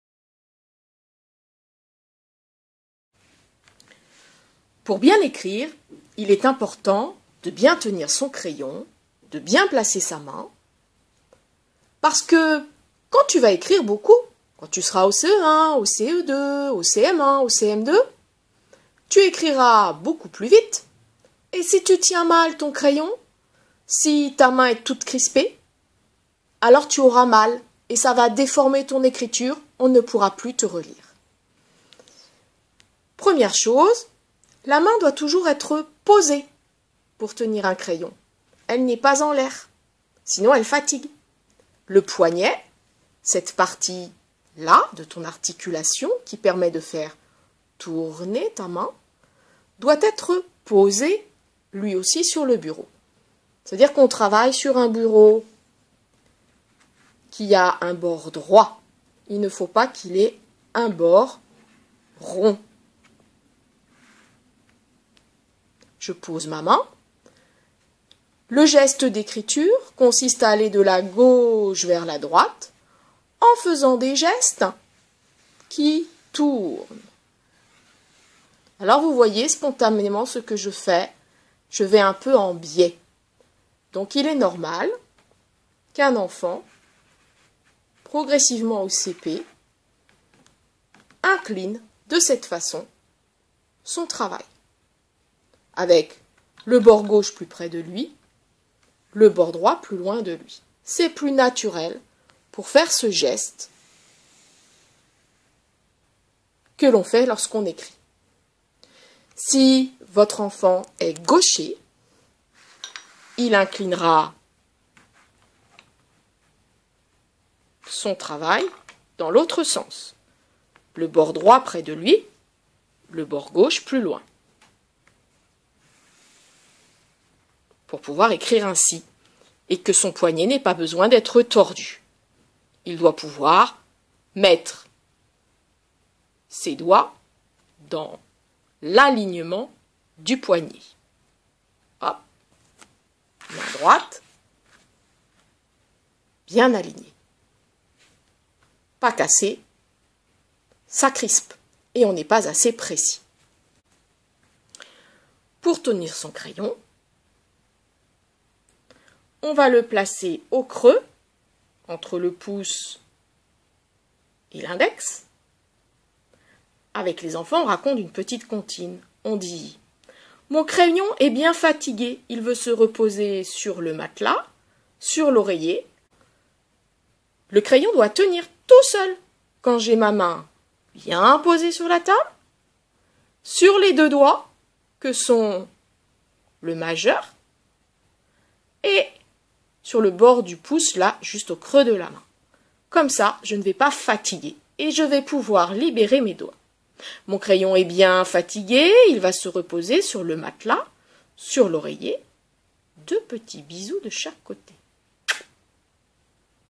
La tenue du crayon chapitre1
Placement de la main, du poignet, de la feuille et tenue du crayon.